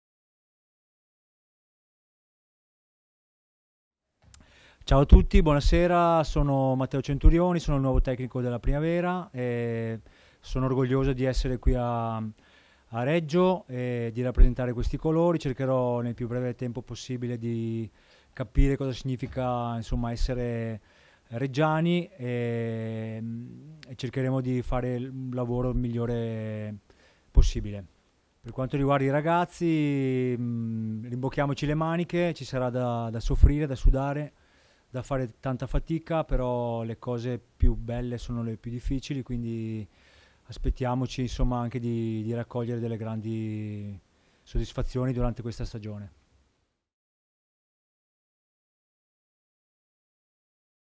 MATTEO CENTURIONI E' LA GUIDA TECNICA DELLA PRIMAVERA GRANATA
Le prime parole di saluto alla piazza granata di Matteo Centurioni, allenatore al quale è stata affidata la guida tecnica della formazione Primavera.
Dopo una carriera ventennale da calciatore professionista, Centurioni ha iniziato il proprio percorso da allenatore lavorando da subito nel mondo dei settori giovanili.